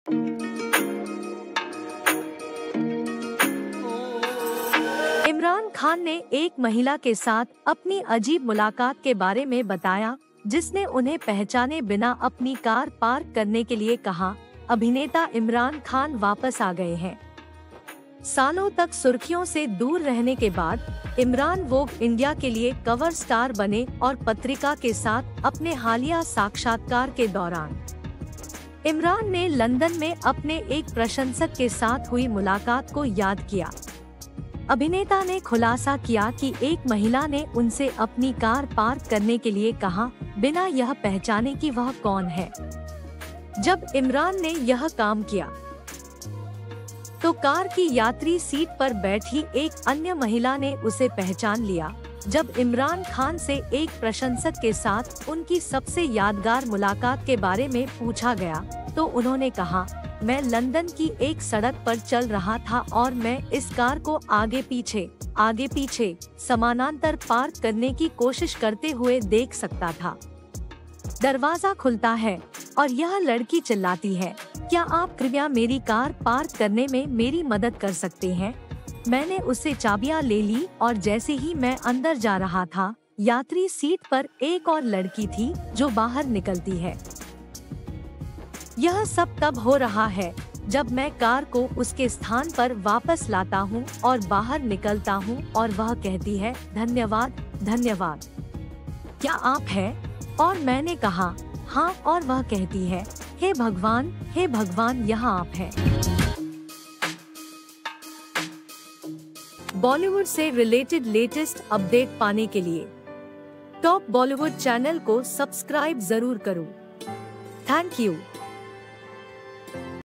Imran Khan Recalls Unusual Encounter With A Fan In London, Who Asked Him To Park Her Car
"Top Bollywood"